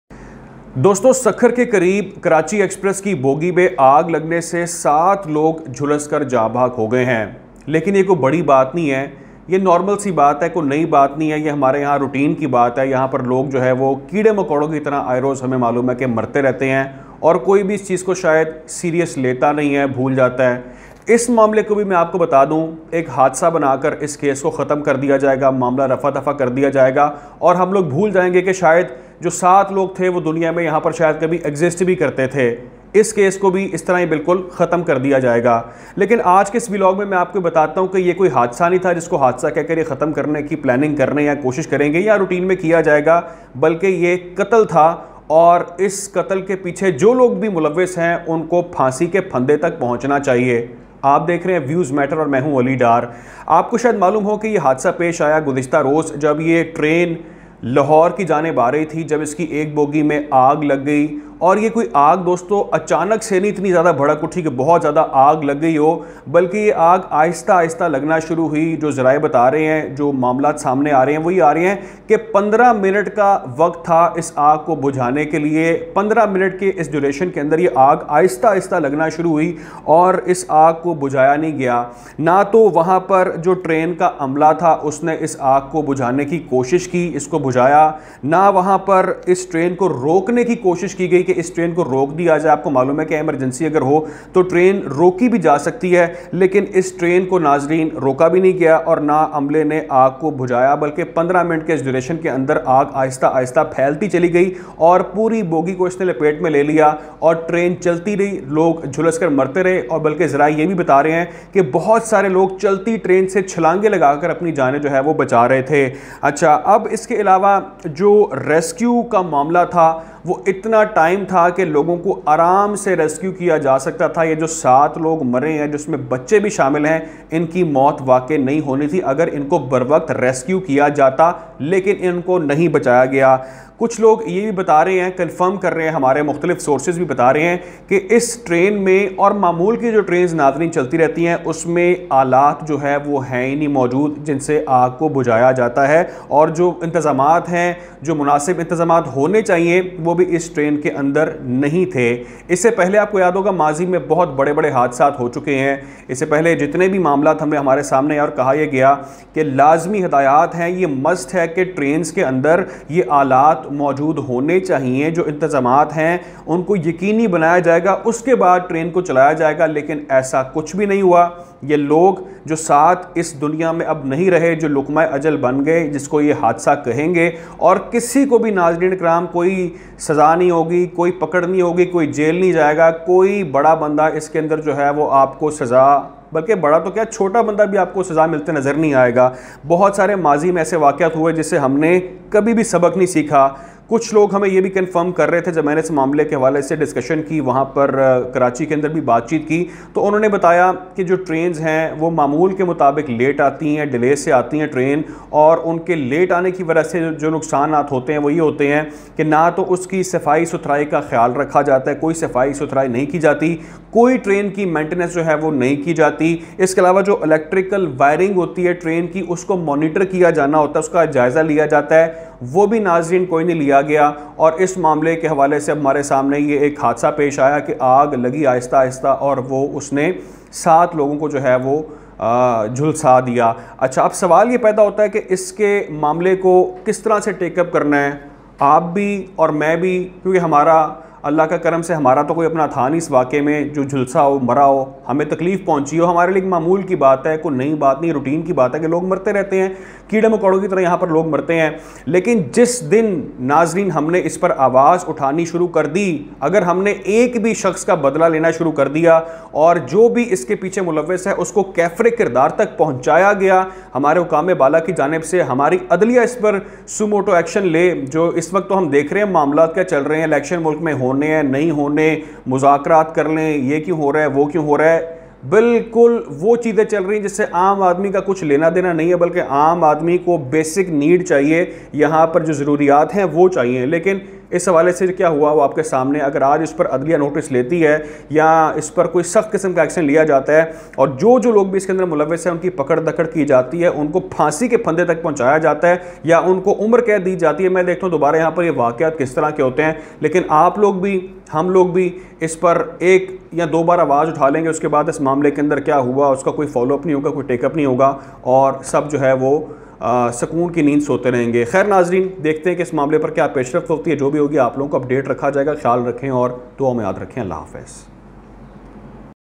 کیا یہ کیڑے مکوڑے تھے ؟ 7 پاکستانی جھلس گئے کوئی پوچھنے والا نہیں، کئی گھروں کے چراغ گل ہو گئے
سکھر ٹرین حادثہ، کئی گھروں کے چراغ بچھ گئے ، کئی بچے اپنے والدین کے انتظار میں ہیں ، اور ہمیشہ رہیں گے ، لیکن حکمرانوں کو کیا ان کے بچے تو دوسرے ممالک میں عیاشی کر رہے ہیں

سکھرٹرین حادثہ